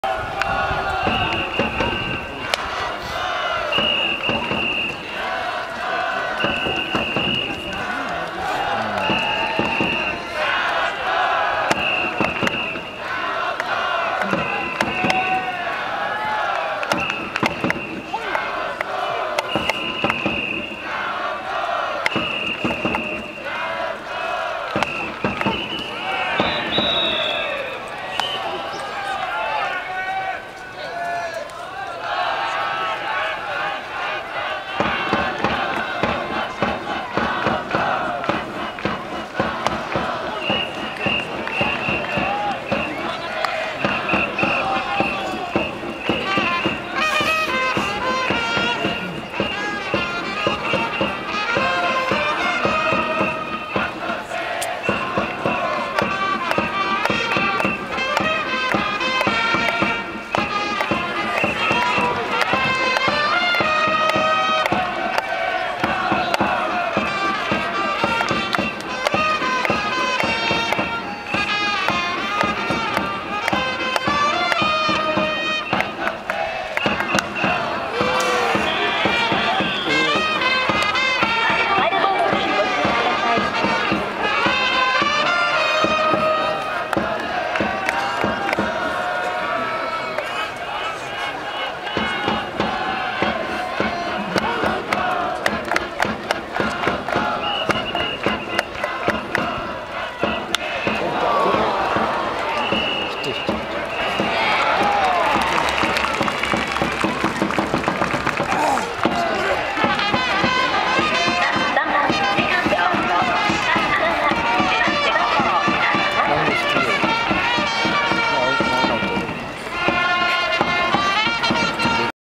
2012/3/3 下柳剛 vs 渡辺直人 下柳 楽天初登板
下柳の故郷・長崎ビッグＮスタジアムで行われたオープン戦

楽天移籍後初登板